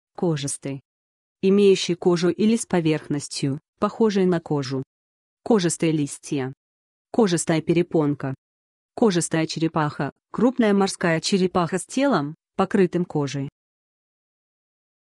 КОЖИСТЫЙ - что это такое? значение и описание
Хочешь знать что значит КОЖИСТЫЙ? тогда слушай до конца.

 -ая, -ое. Имеющий кожу или с поверхностью, похожей на кожу. Кожистые листья. Кожистая перепонка. Кожистая черепаха (крупная морская черепаха с телом, покрытым кожей).